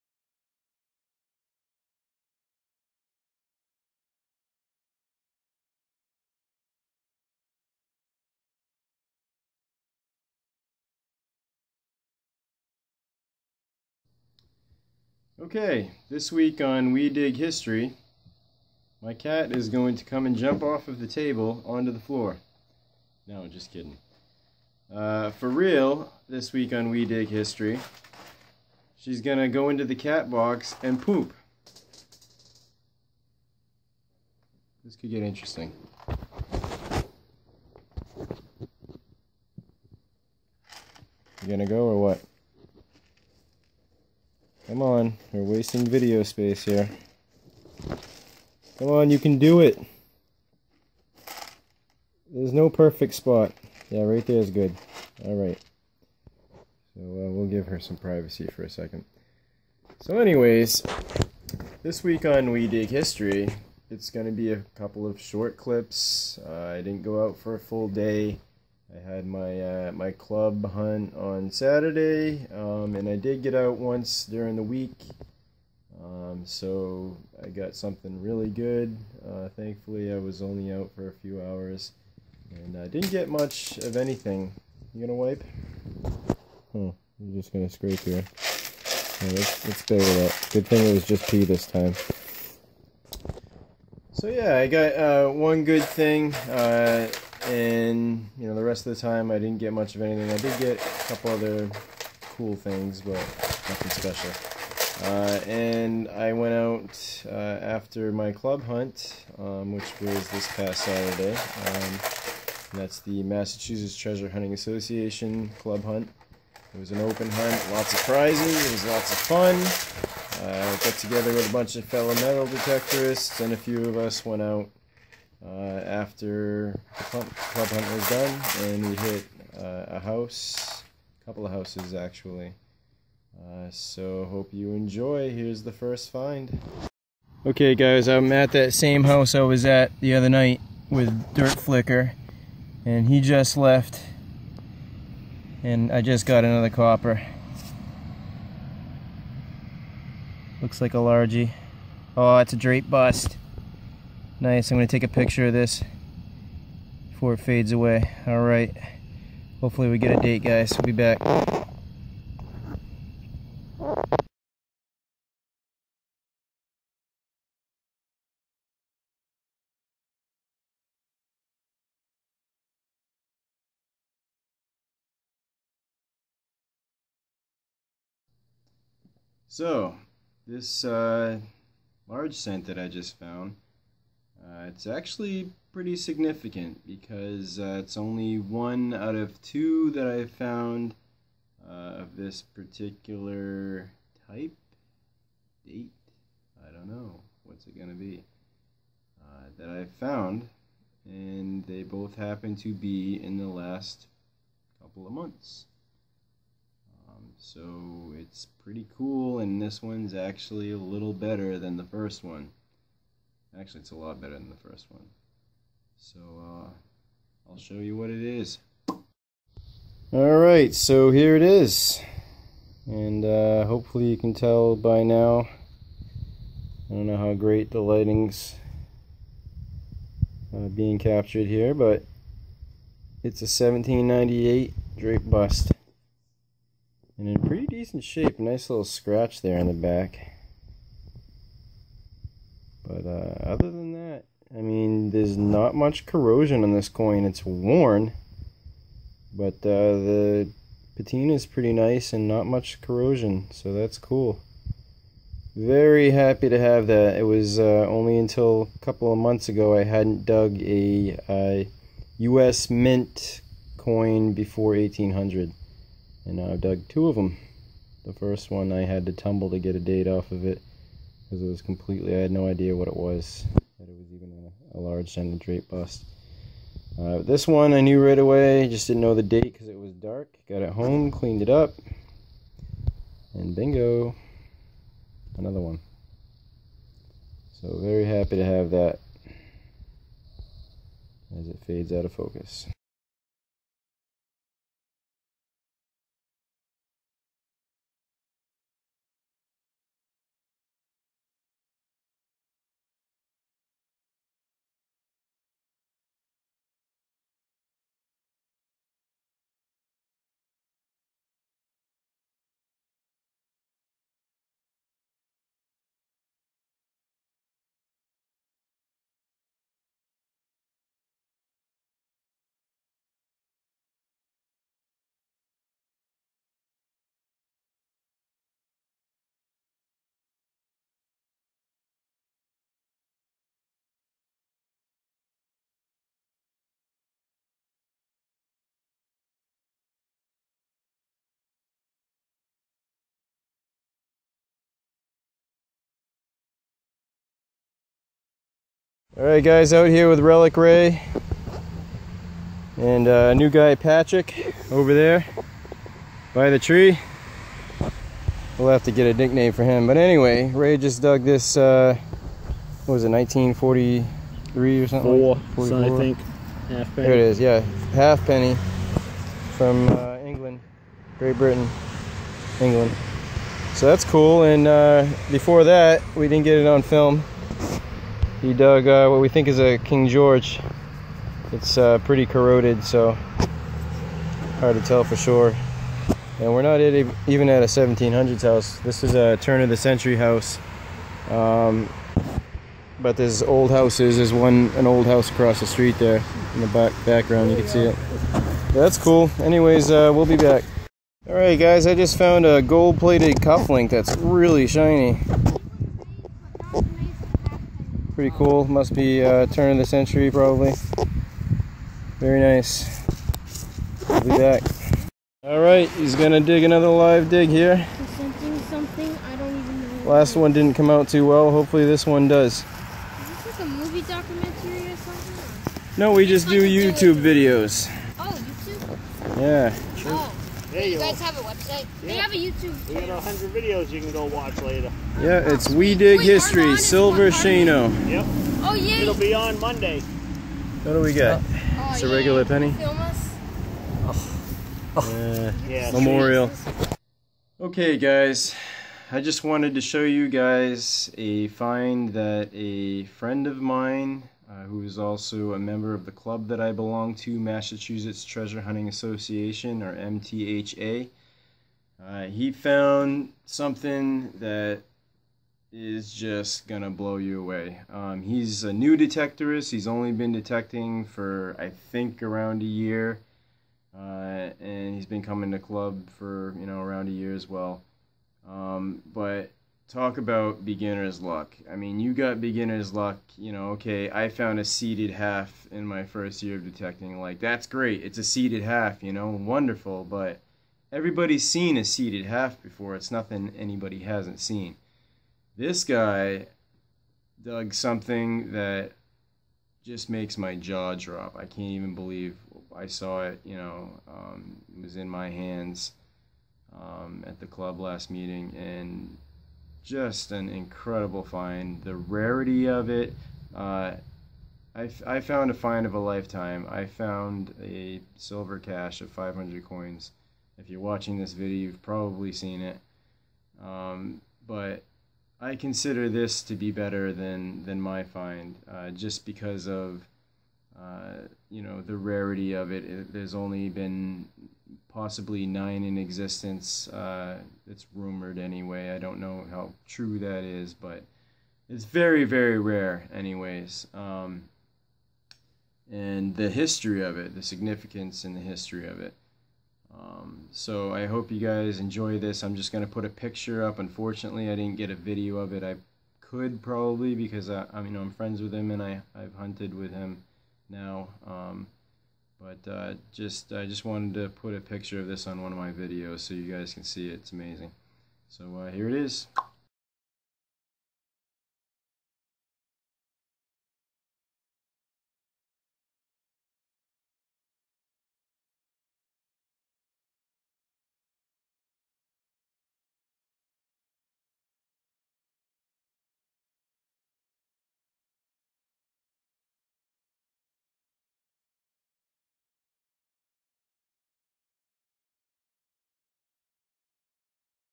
Short hunts + Extremely Rare Spectacular Historical Military Artifact!!!
Find of a Lifetime for a friend of mine, and a very nice large cent for me.